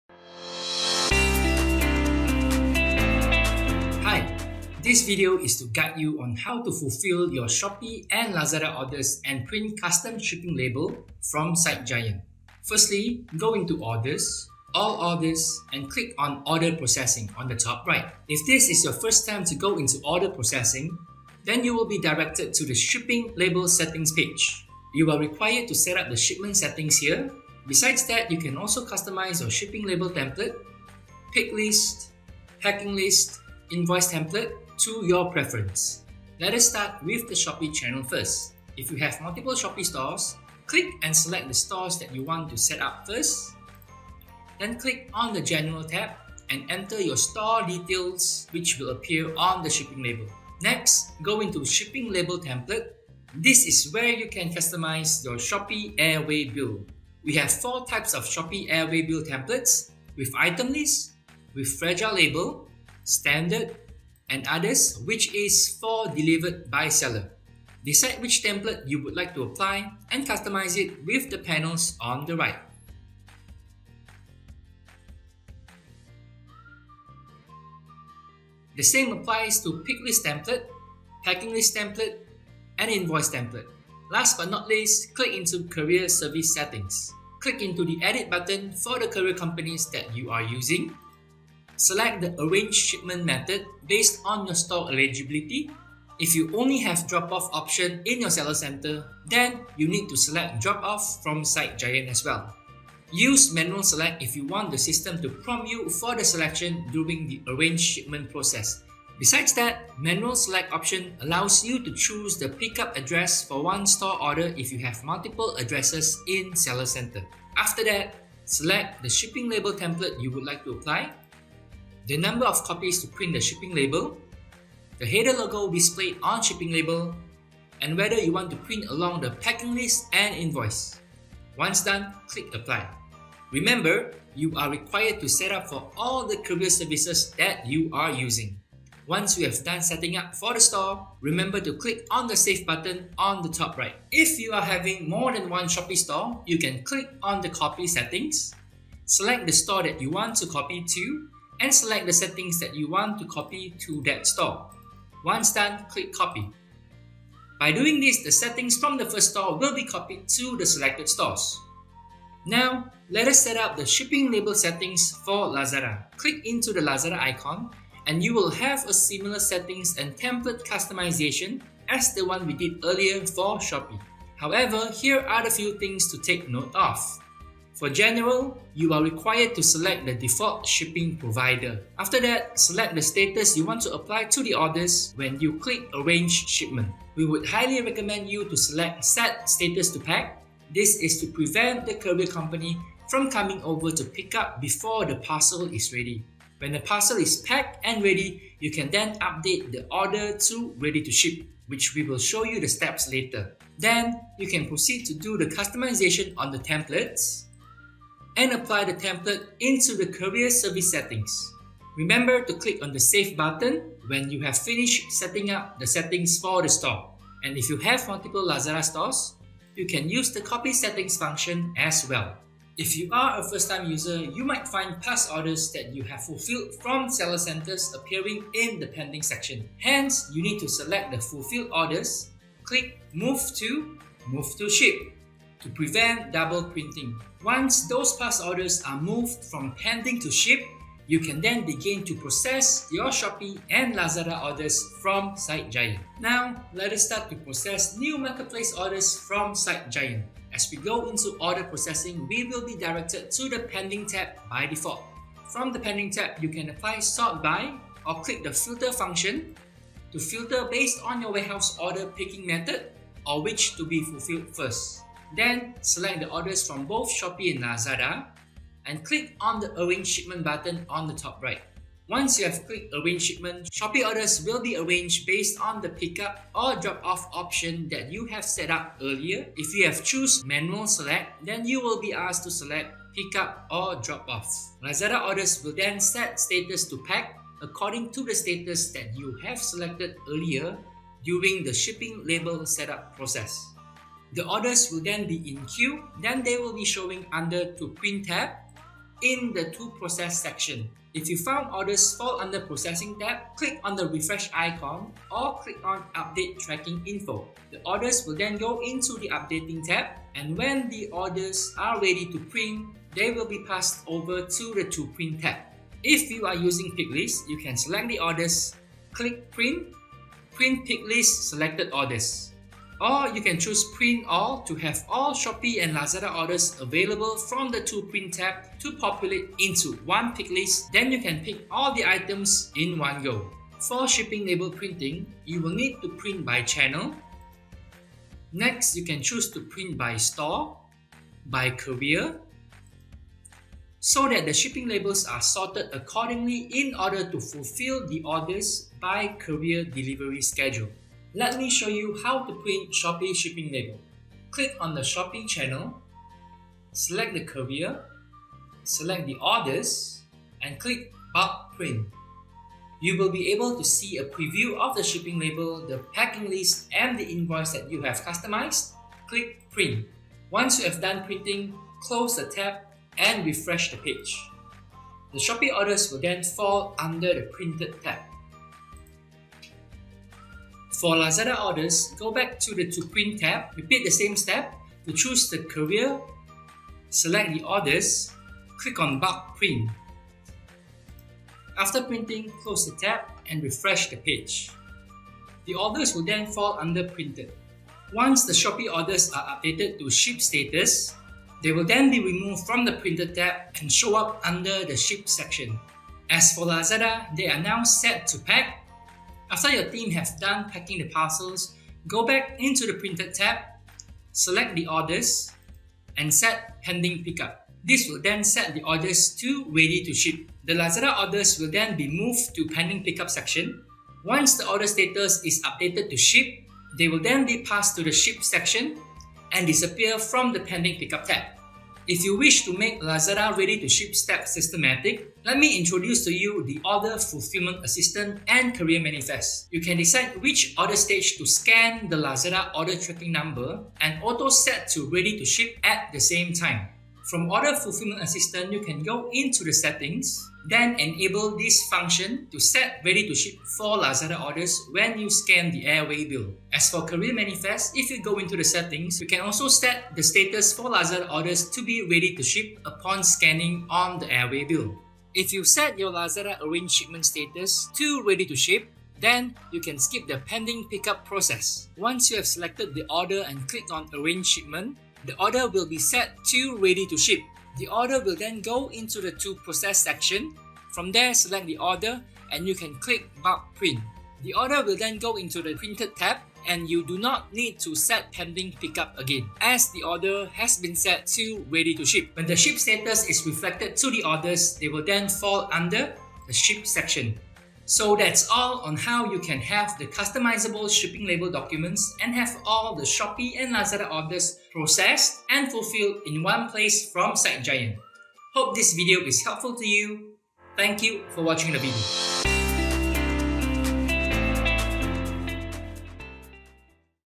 SiteGiant Order Processing
Do note that this video is a tutorial for SiteGiant merchants.

For this episode, we will be covering how to fulfill your Shopee and Lazada orders and print custom shipping labels from SiteGiant.

0:36 Setting up your Shipping Label Setting and customize airway bill templates for Shopee store
2:24 How to copy existing shipping label settings to your other Shopee stores
2:42 Setting up your Shipping Label Setting and customize airway bill templates for Lazada store
3:42 How to move fulfilled orders to Shipped Tab for first-time user
4:06 How to process your Shopee and Lazada orders from SiteGiant
5:16 How to print pick list
5:51 How to print Shopee shipping labels
6:21 How to print Lazada shipping labels
6:46 How to set “Ready To Ship” for Lazada orders
7:12 How to set Lazada “Ready To Ship” step automatically by using Order Fulfillment Assistant and Carrier Manifest

We hope that this episode gives you some insight on how to print customizable shipping label documents and have all Shopee and Lazada orders processed.